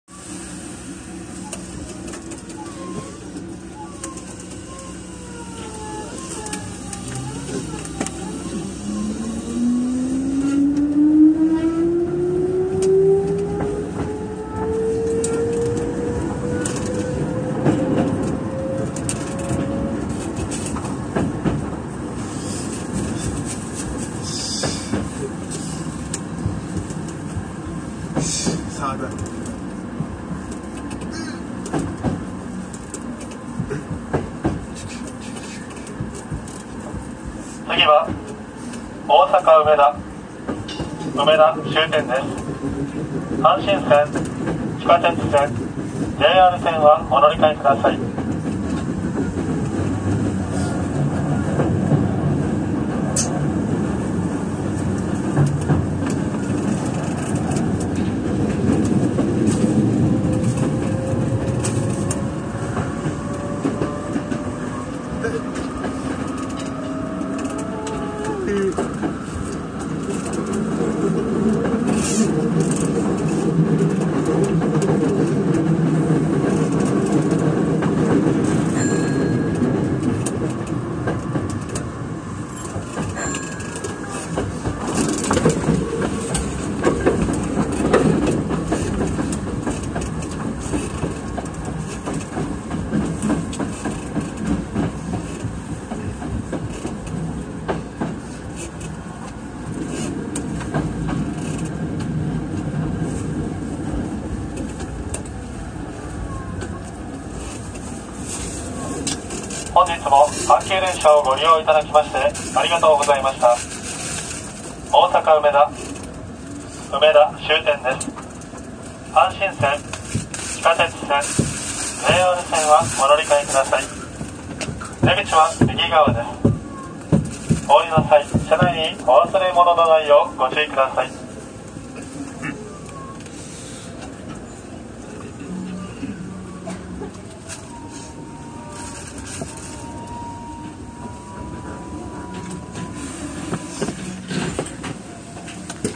【阪急の前面展望】宝塚線 中津→梅田
関西を代表する私鉄の阪急電鉄。

今回は阪急宝塚線の中津（なかつ）から終点の梅田（うめだ）までの前面展望になります。